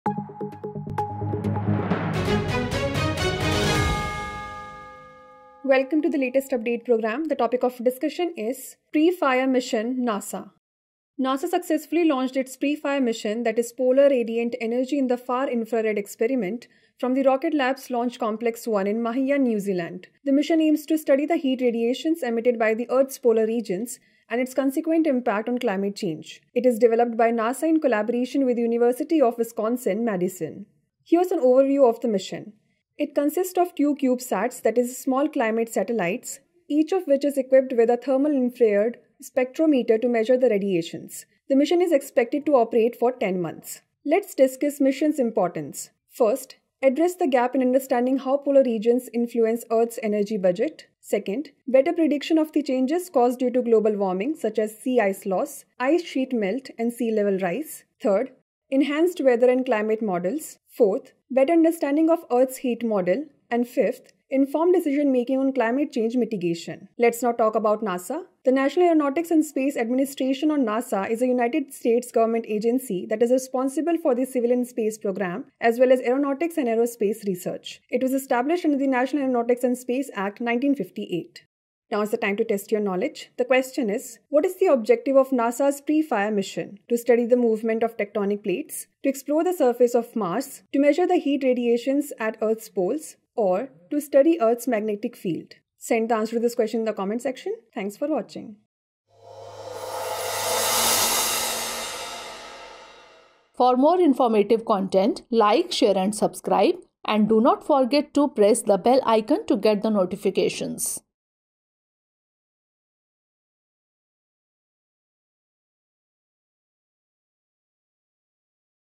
PREFIRE Mission | NASA | Latest Update | Drishti IAS English
NASA launched its PREFIRE mission to study Earth’s Polar heat radiations. This mission consists of two CubeSats equipped with advanced thermal infrared spectrometers to measure the far-infrared heat emitted from the earth’s Polar regions. It will help fill the gaps in understanding Earth’s heat budget and its influence on global climate change.